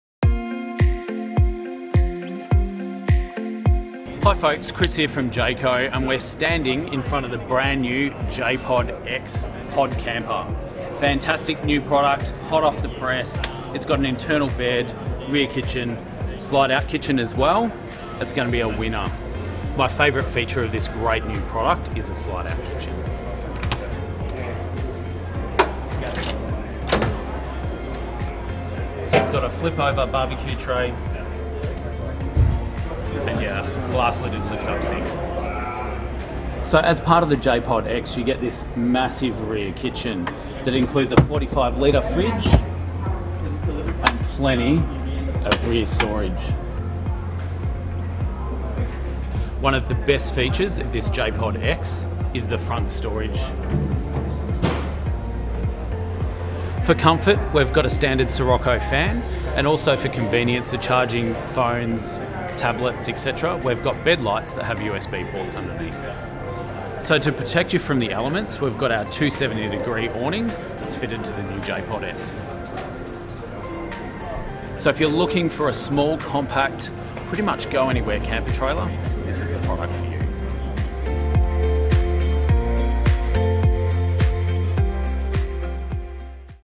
Jayco Sydney J-Pod-X camper
Chris Hooper,  Dealer Development Manager at Jayco Australia, walks you through the brand new J-Pod-X camper. If you are looking for a small compact go anywhere camper then this is the one for you. The J-Pod-X will be on display at all upcoming trade shows including the National 4X4 show in Sydney.

KEEP UP TO DATE WITH JAYCO SYDNEY!

You’ll receive hints, tips and the latest news about the Jayco Sydney range direct to your inbox.